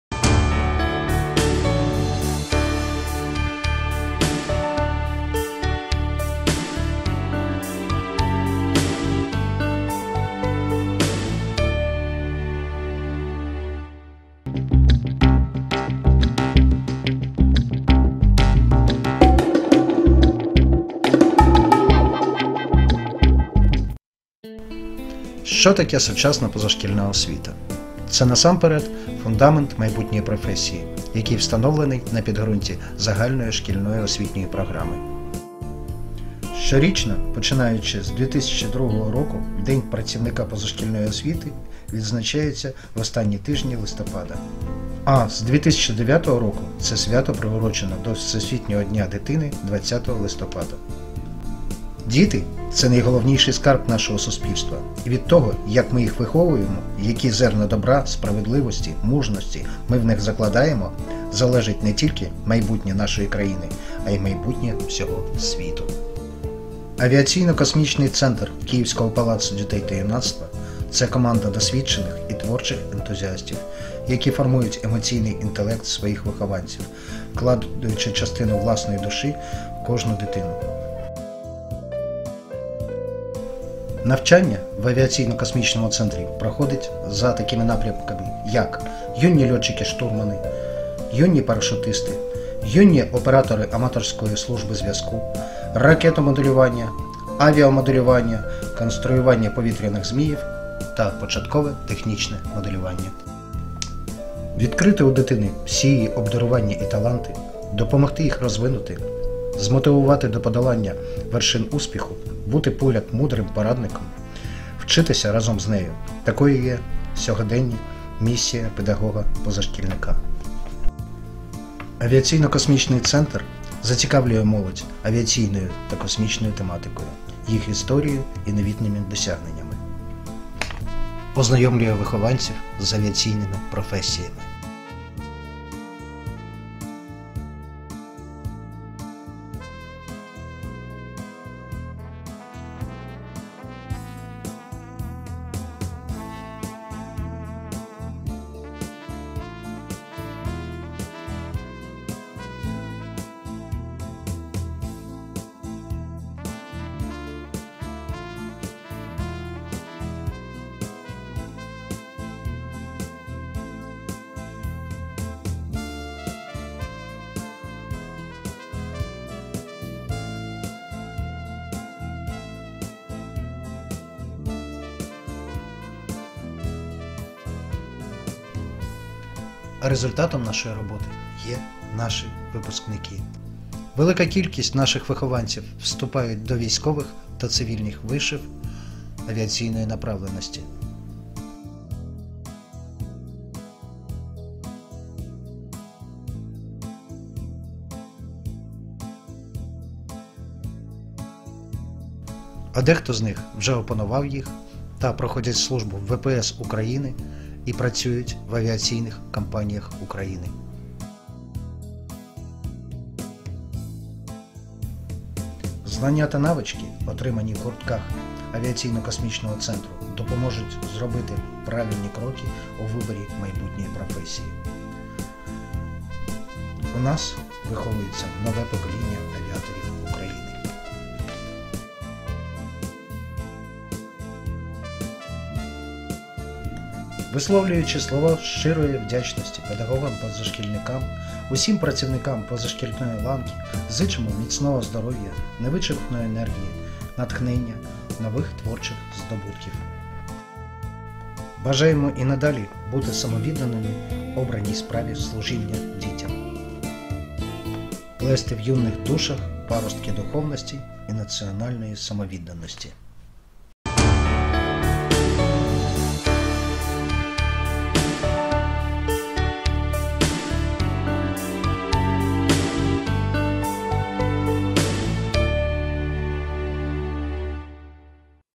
З Днем позашкілля Києва!
Вітання з Днем позашкілля Києва від Авіаційно-космічного центру Київського Палацу дітей та юнацтва.